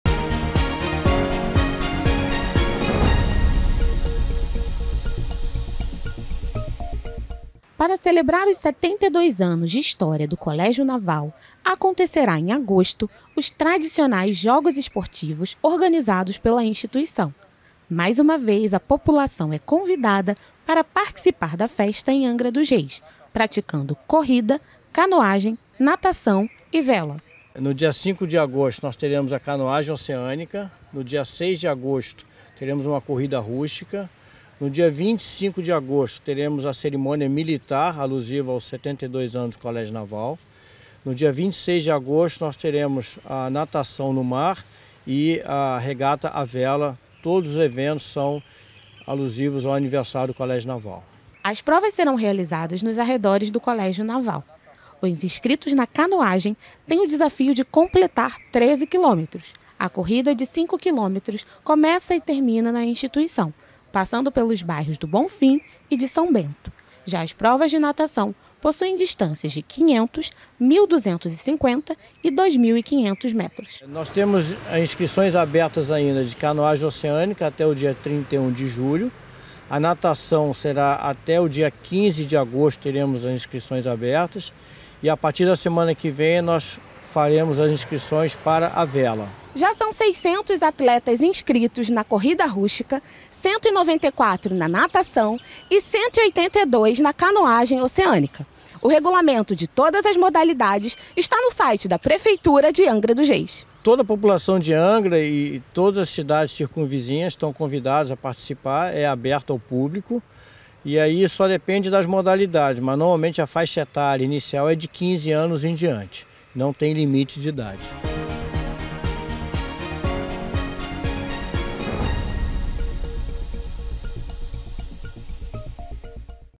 Inscrições abertas para os Jogos Esportivos do Colégio Naval
Provas esportivas de corrida, natação, canoagem e vela vão acontecer em comemoração aos 72 anos do Colégio Naval